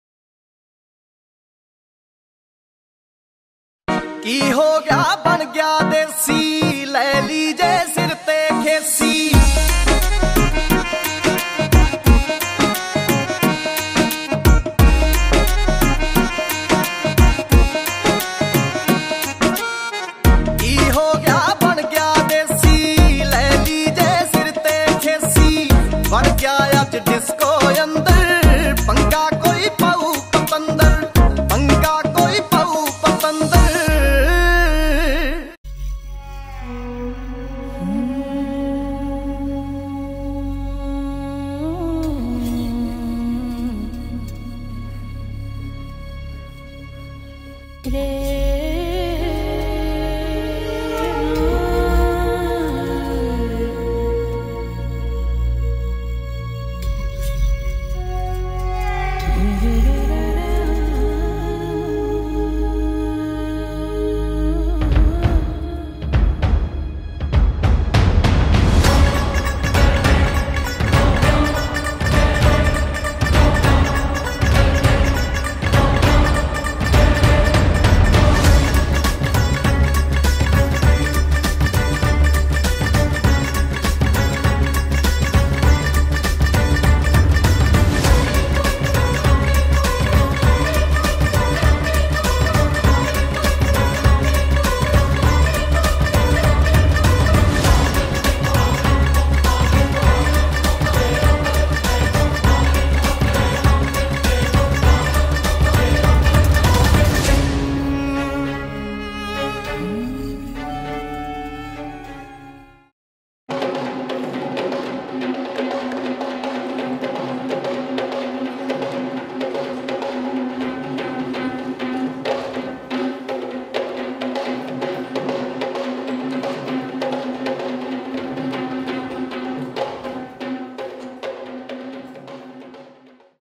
Kandy (Sri Lanka)
Kandy, kulturowa stolica i drugie co do wielkości miasto Sri Lanki.